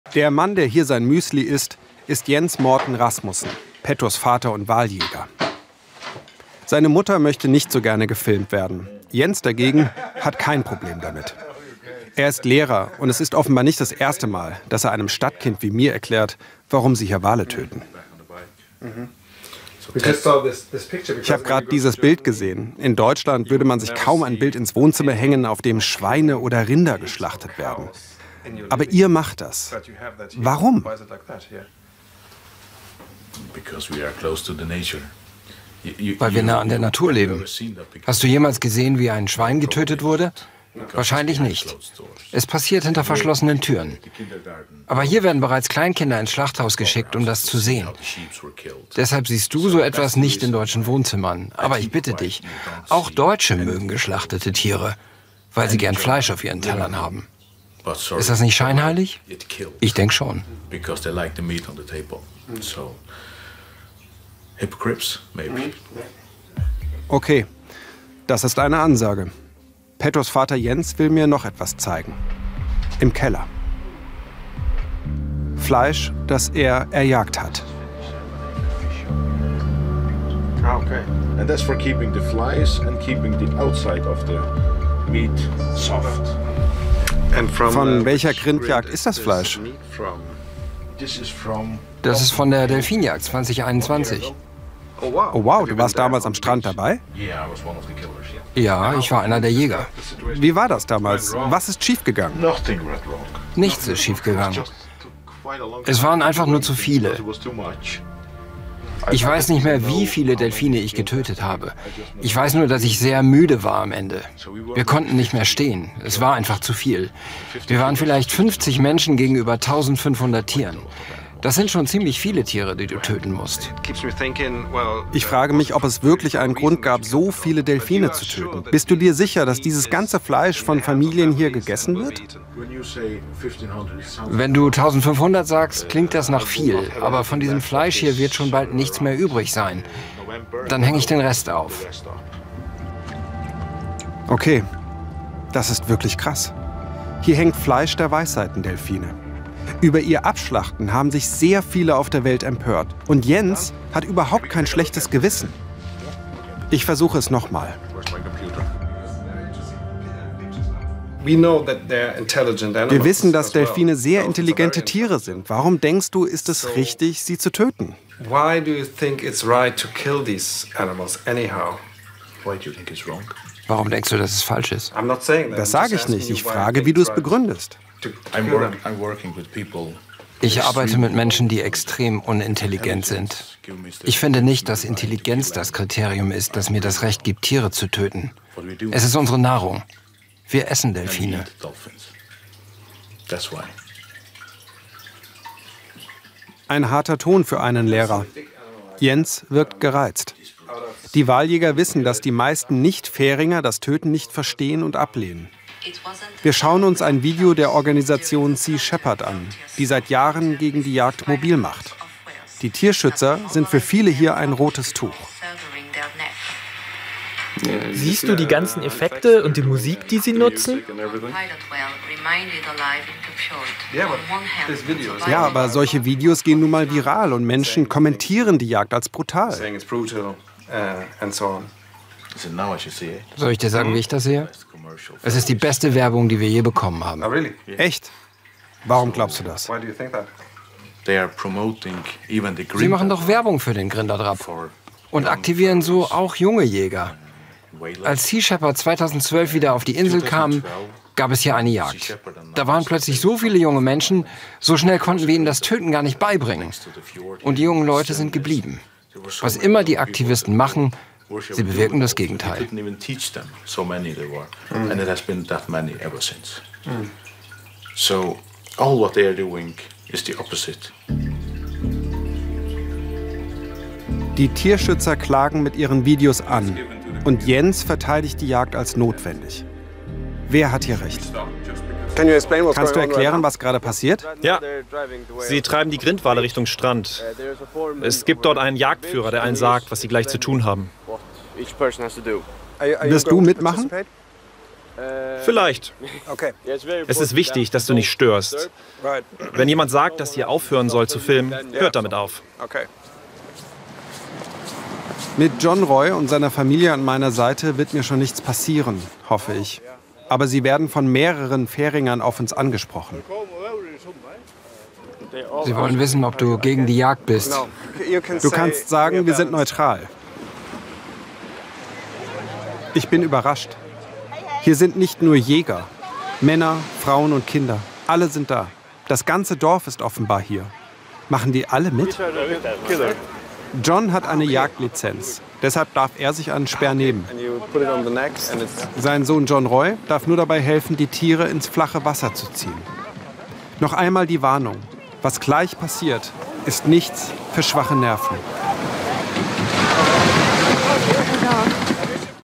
Warum auf Färoer Delfine getötet werden
🎥 Waljagd auf den Färöer Inseln - Warum gibt‘s das noch? / Doku / 45 Minuten

Wale und Delfine - fast überall auf der Welt begeistern sie uns. Sie gelten als intelligent, sozial und die Begegnung mit ihnen berührt diejenigen, die sie erleben dürfen. Aber es gibt auch Menschen in Europa, die Wale und Delfine töten. Beim so genannten Grindadráp, der Wal-Treibjagd, werden auf den Färöer Inseln jedes Jahr hunderte Grindwale und andere Delfine abgeschlachtet.

Wenn dir unser Channel hier schon gefällt, wirst du unsere anderen Social-Kanäle lieben: